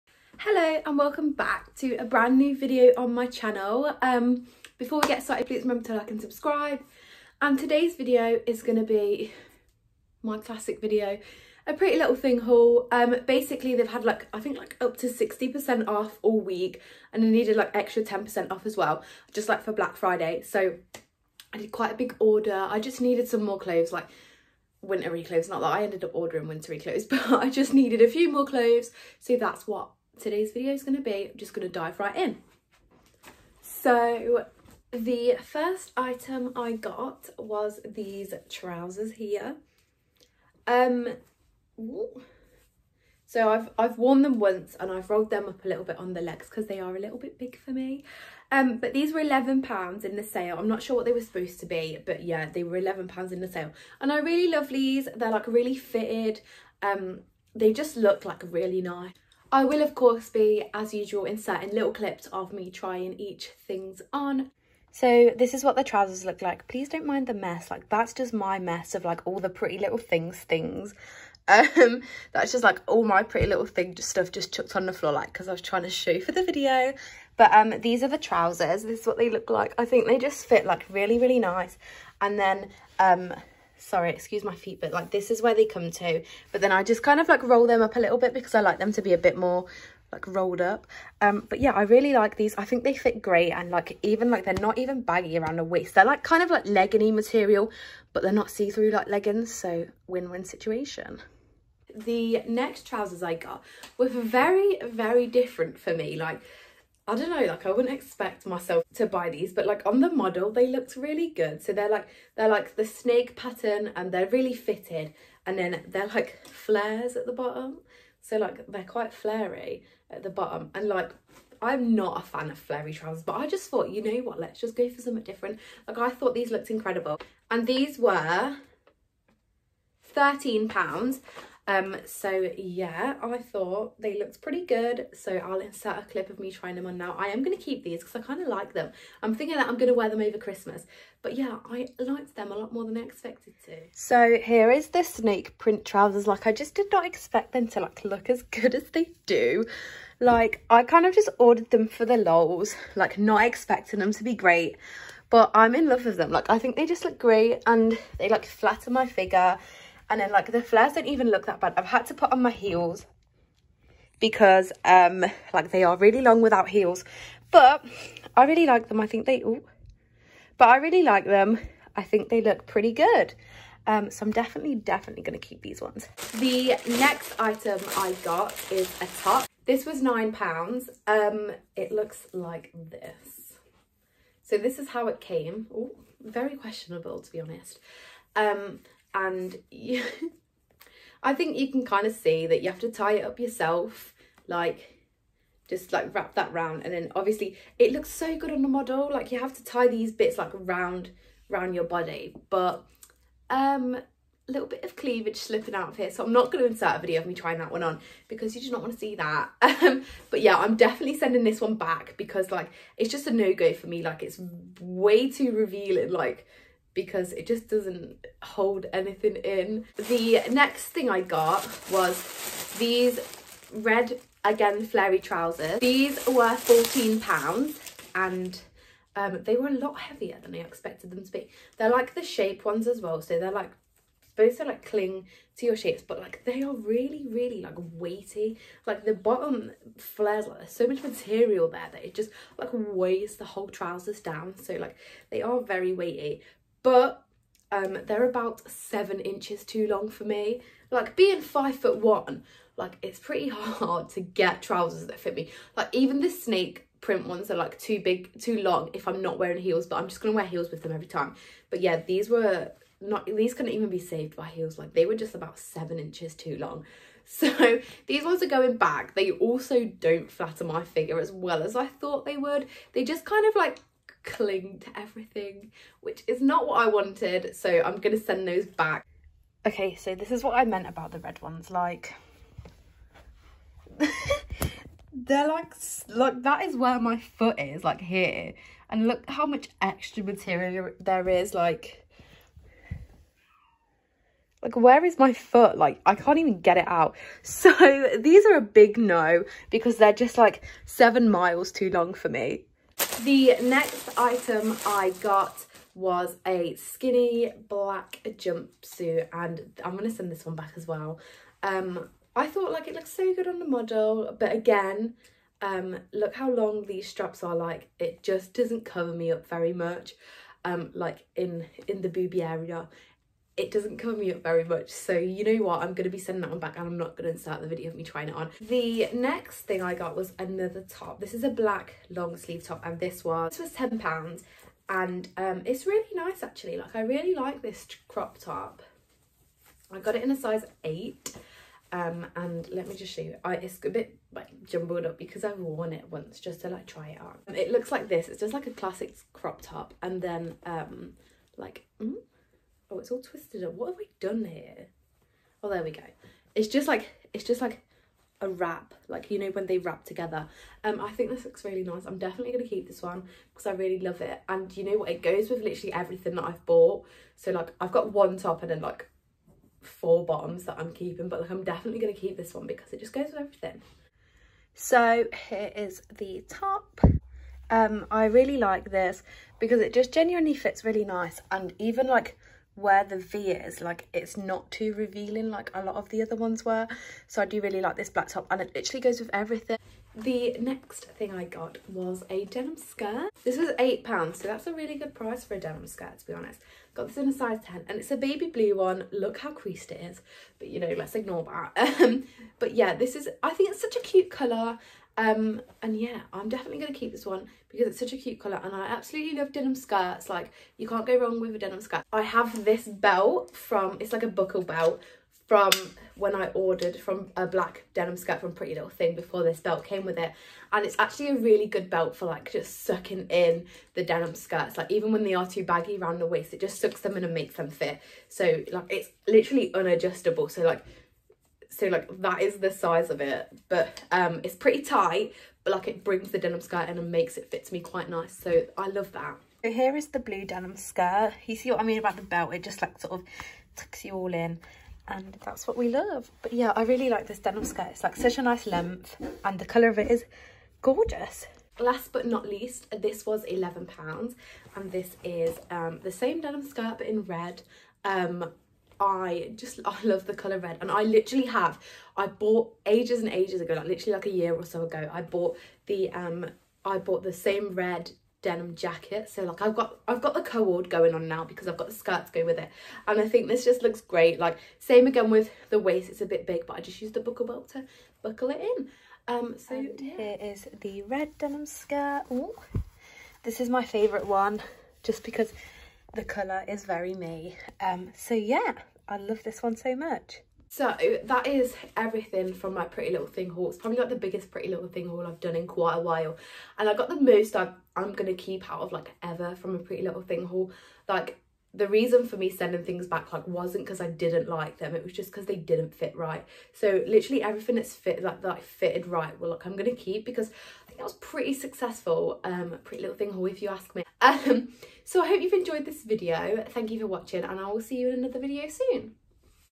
WINTER 2019 PRETTY LITTLE THING HAUL !
Hello my name is Courtney and welcome back to another video on my channel where today I show you my large haul from Pretty Little Thing for winter 2019!